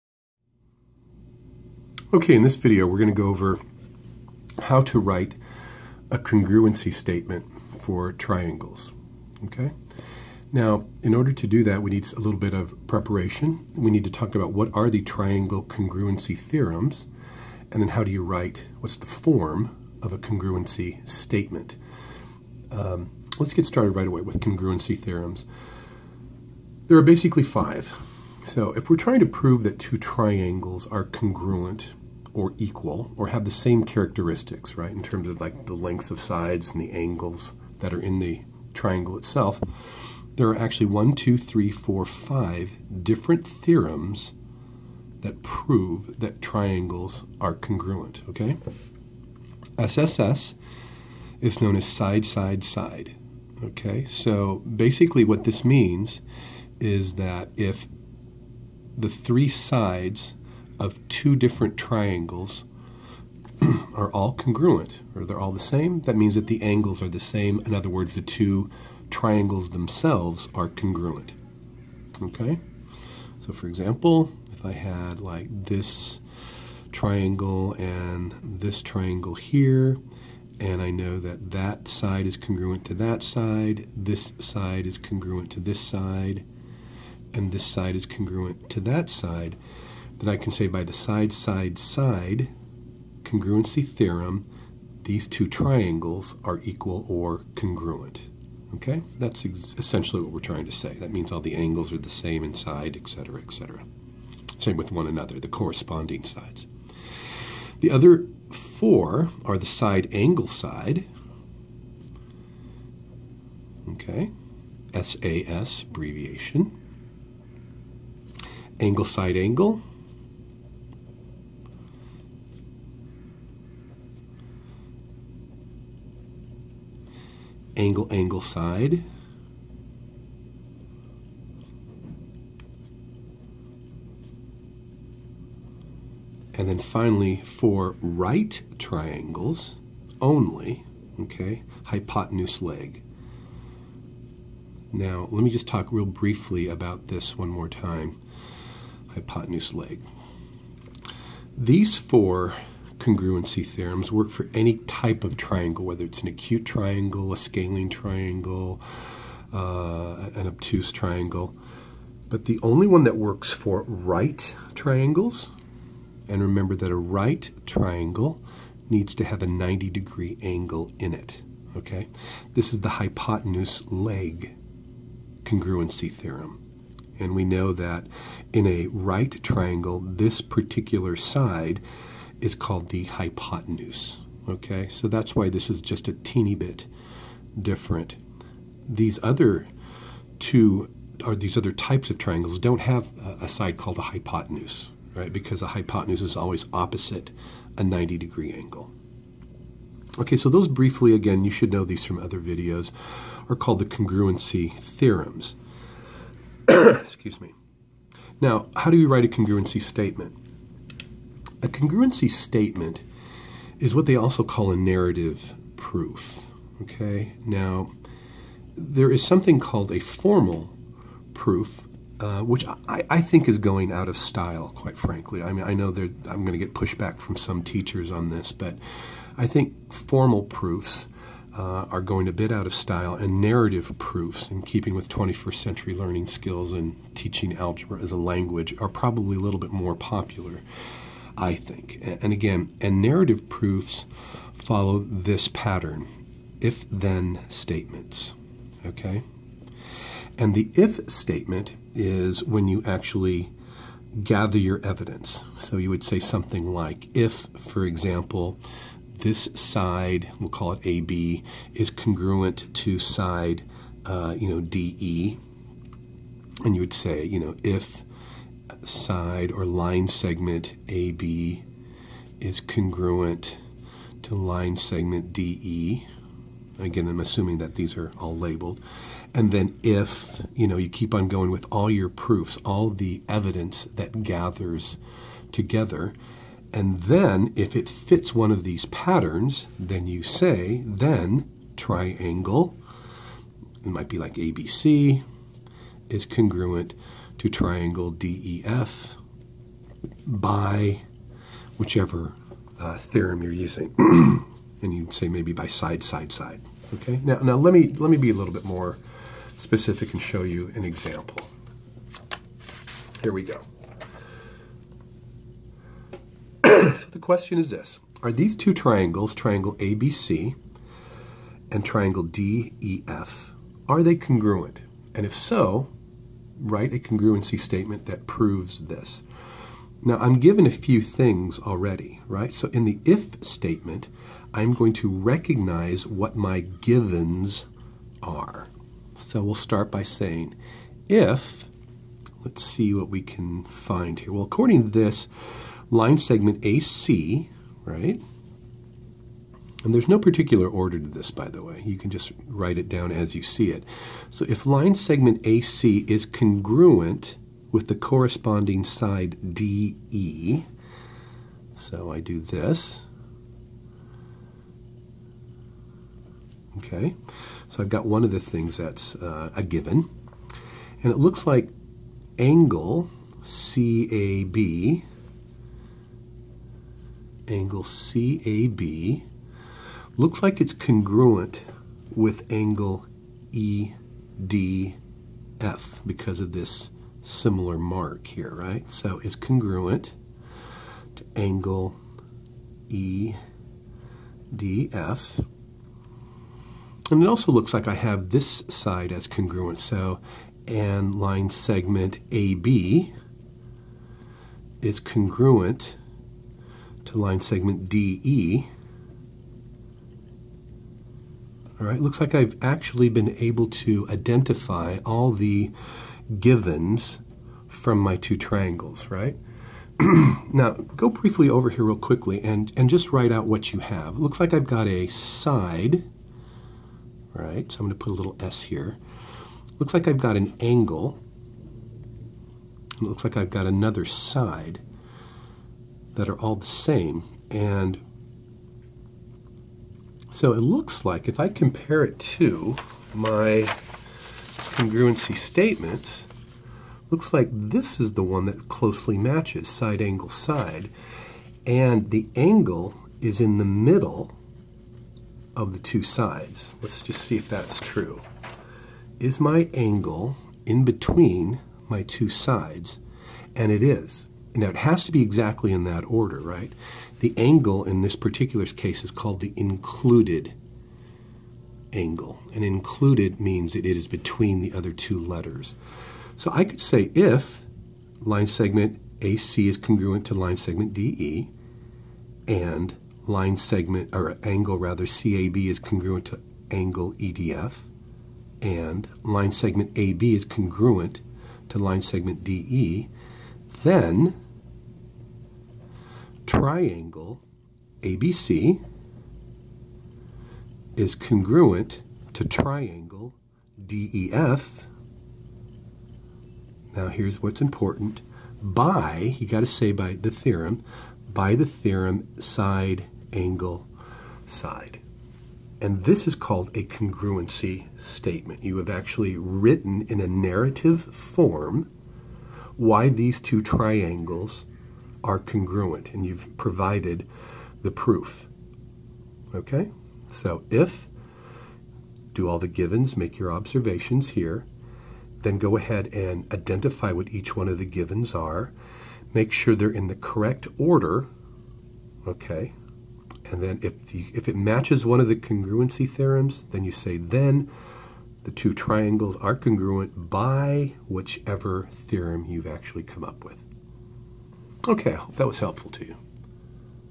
Triangles: How to Write a Triangle Congruency Statement
This video is a demonstration of how to write a narrative proof to demonstrate that two triangles are congruent.